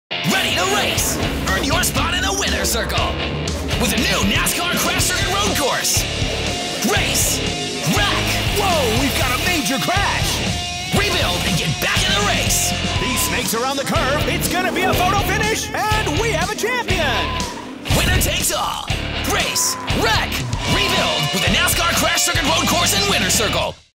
Far Out Toys TV Ad NASCAR Crash Circuit Road Course
Far Out Toys provided their TV ad for the NASCAR Crash Circuit Road course. Enjoy!!!